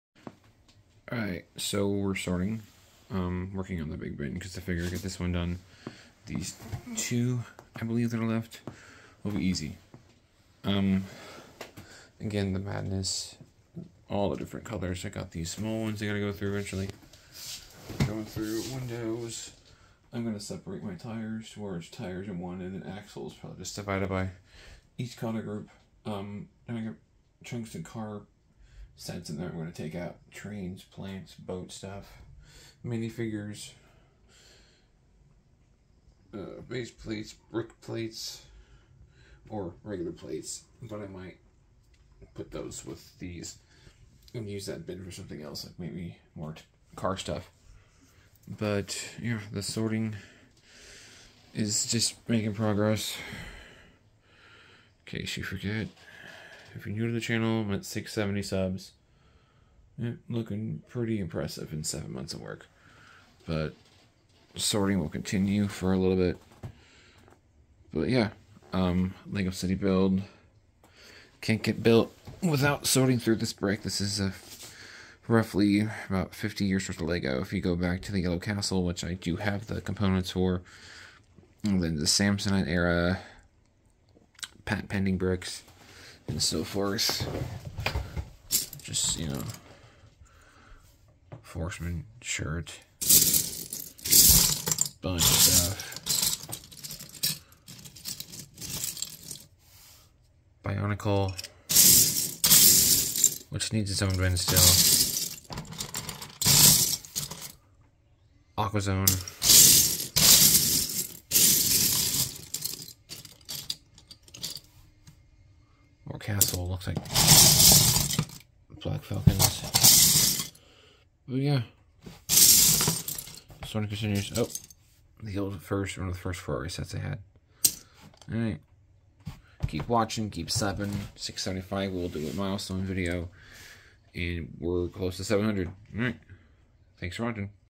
Lego Sorting: Between Builds (5/5/24)
Just sorting through the rest of my bulk brick before getting back to my builds